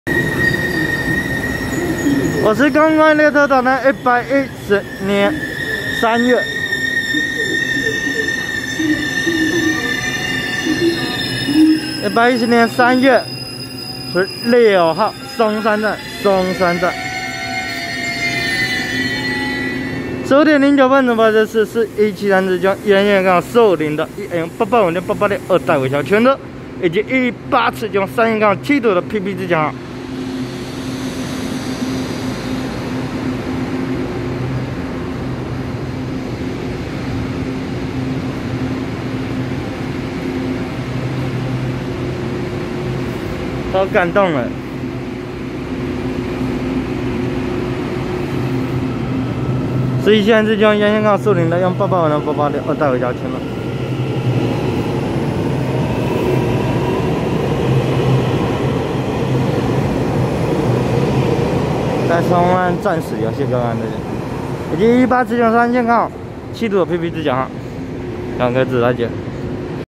4173次經由宜蘭線開往樹林的EMU885+EMU886二代微笑號區間車，和118次經由山線開往七堵的PP自強號
110/3/16松山站15:09分拍攝4173次經由宜蘭線開往樹林的EMU885+EMU886二代微笑號區間車，和118次經由山線開往七堵的PP自強號